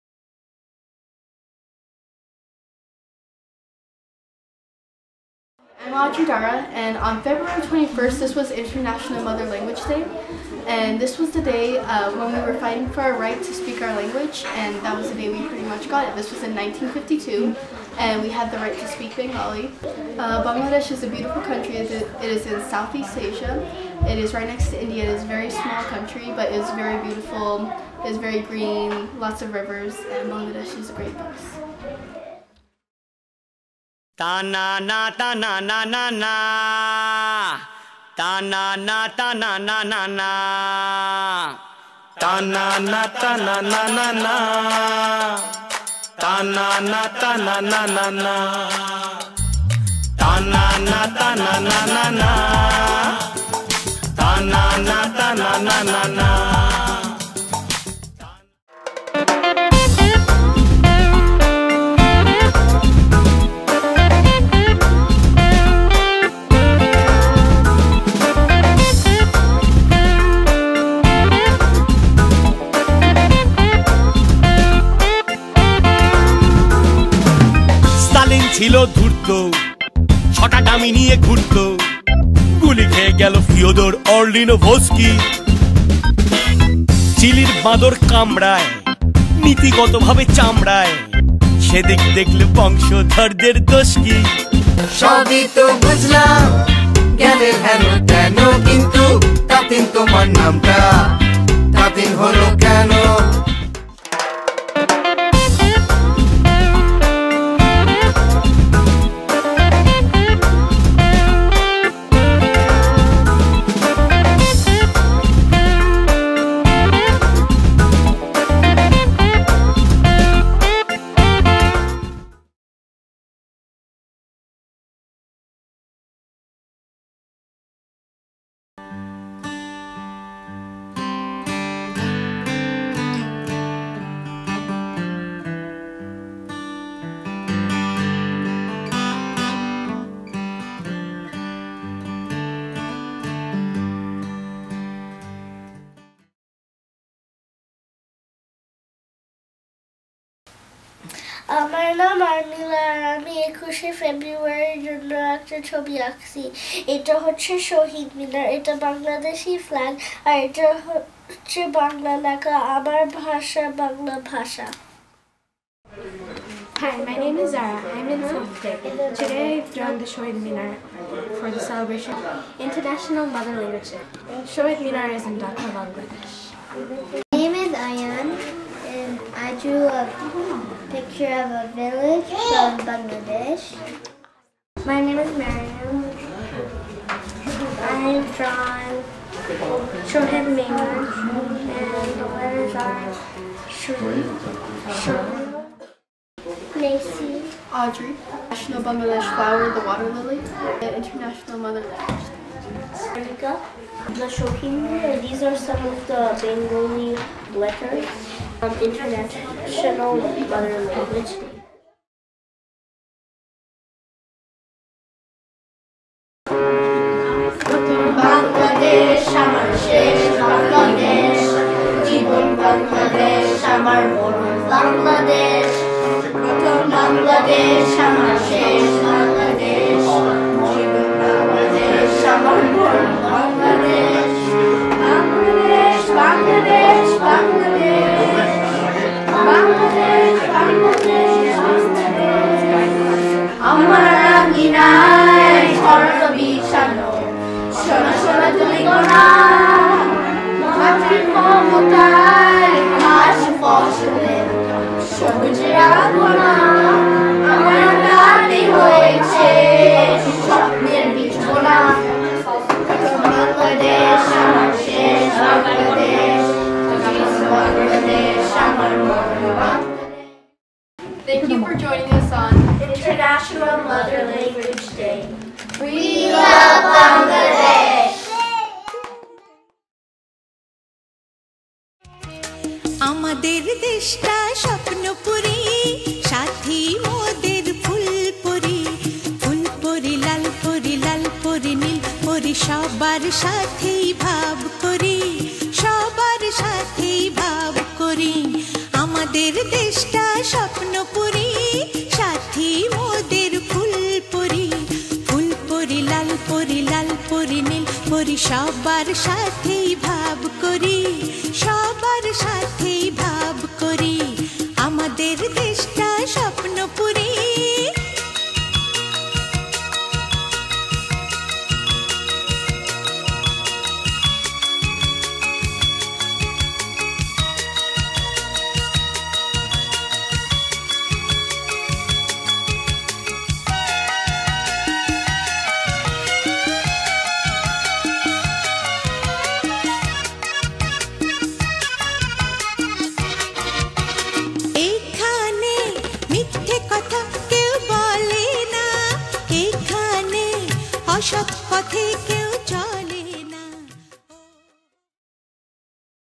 Kids preparing for IMLD (Ver1) 2016
Small video showing the SLC Bangladeshi Community kids participating in the International Mother Language Day (02/21/2016)

The video was recorded on 02/14/2016